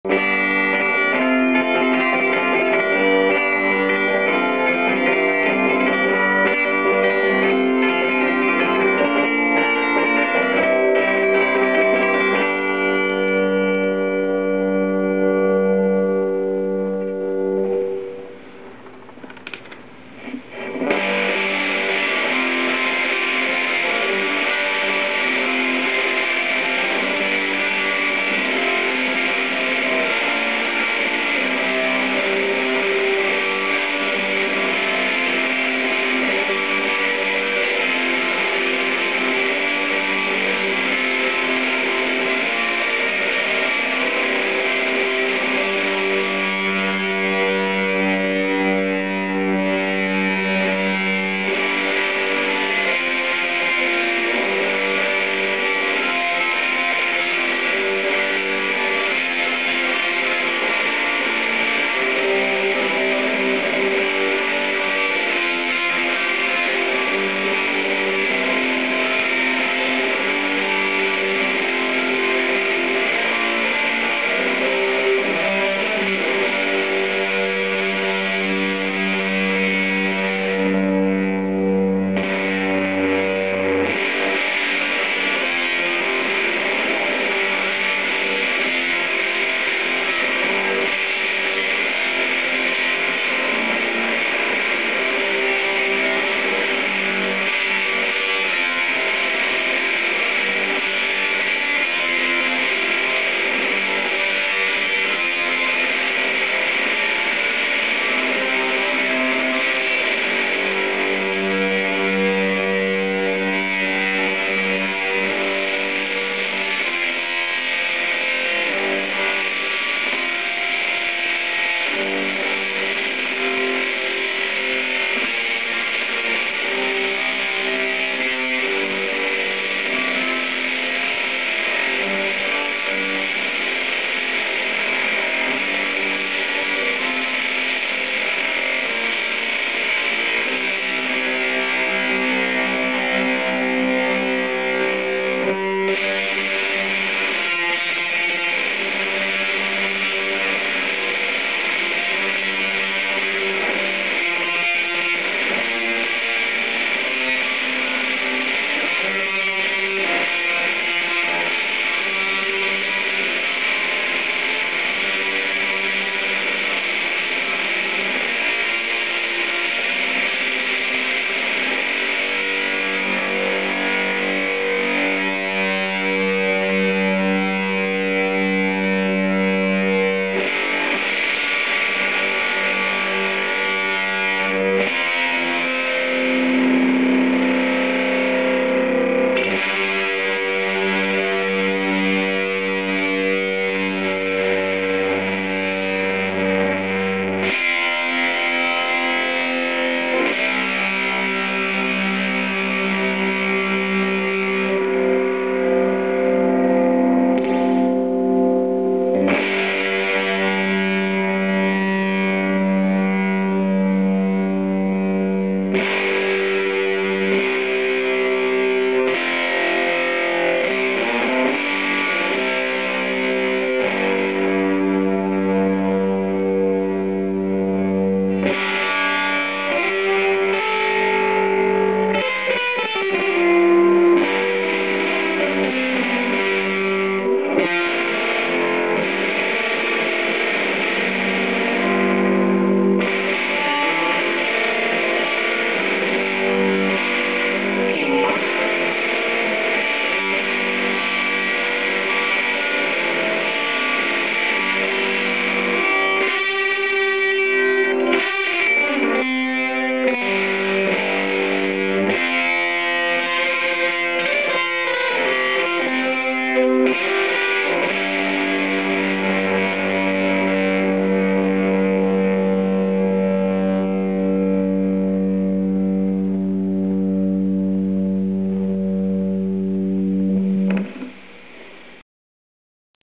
tiki man
Cheesecats demoing a Fuzzhugger Arc Flash.  Papoose SP1 (active humbucker) through a Fender Deluxe 85.  Delay from track offset in Vegas Pro.